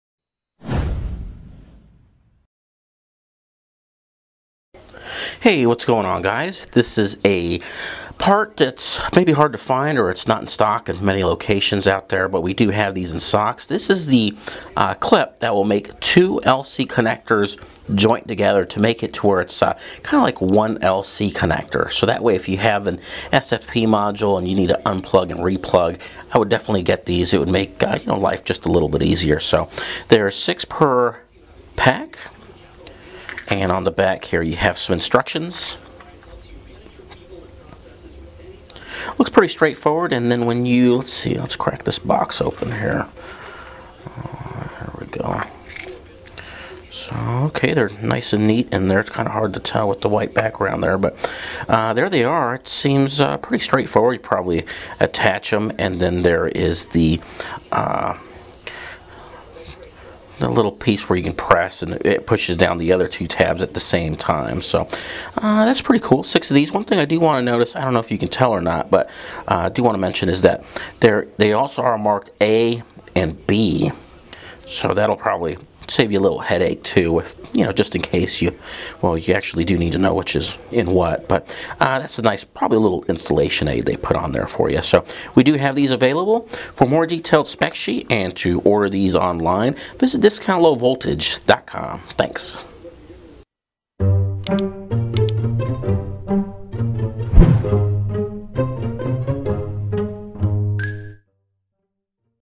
LC Duplex Fiber Optic Connector Clip by AFL
installation convenience, when plugging into an SPF in the switch or for duplex LC couplers, attach this clip to two individual LC connectors then it'll have the feel of one connector. Plug and unplug 2 strands at the same time conveniently.